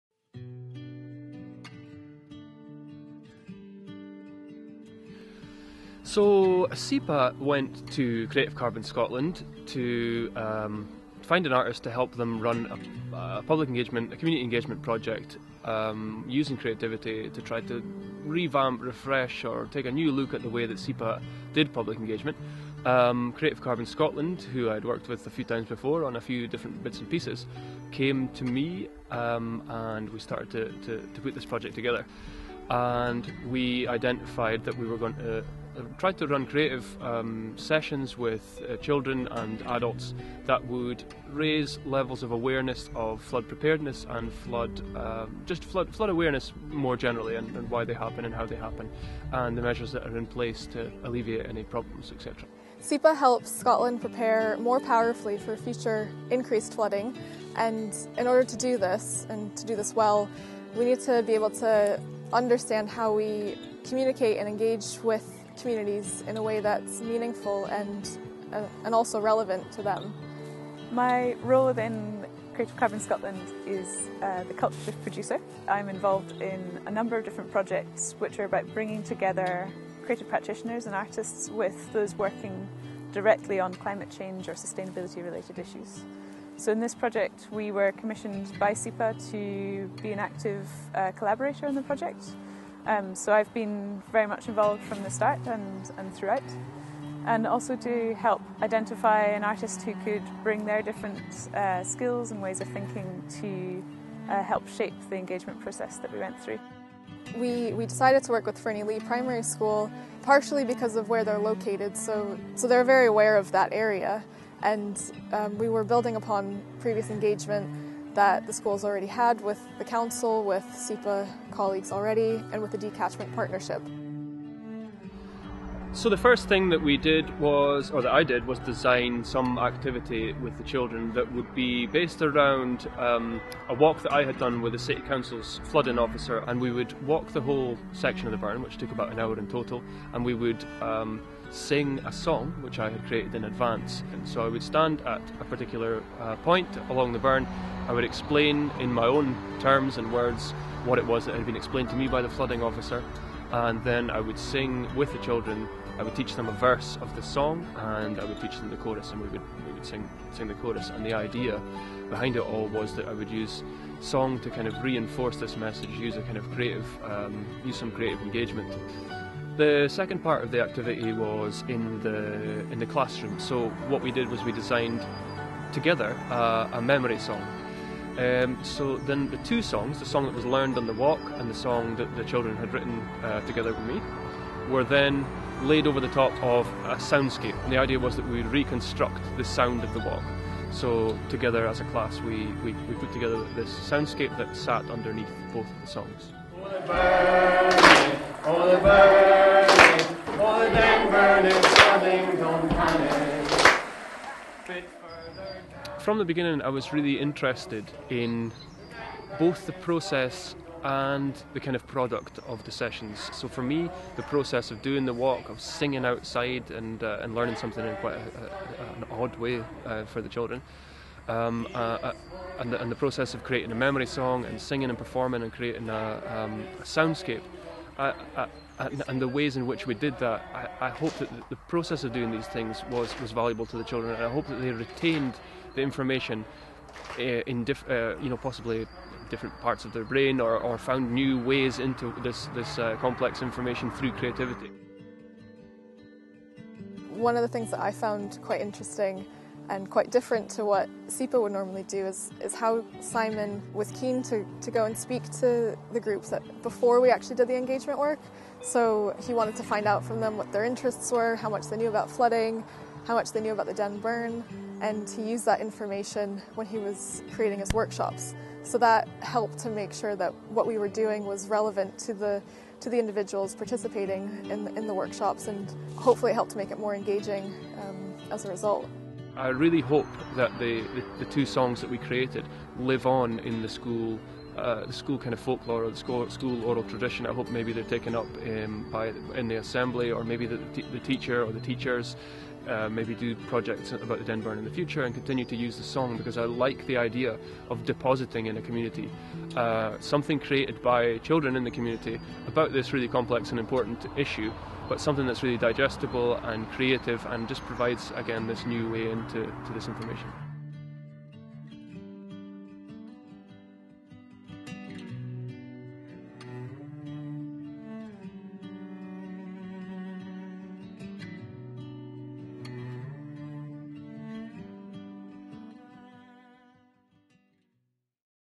Taking a Creative Approach to Flood Awareness
Over winter 2018-19, SEPA and Creative Carbon Scotland tested a new approach to engaging flood risk communities. Unlike traditional forms of public engagement, this project sought the skills and expertise of a creative practitioner to design and facilitate a number of engagement workshops on flooding and local flood risk management in Aberdeen.

Creative Carbon Scotland is a Scottish Charitable Incorporated Organisation, Registered Charity number SC042687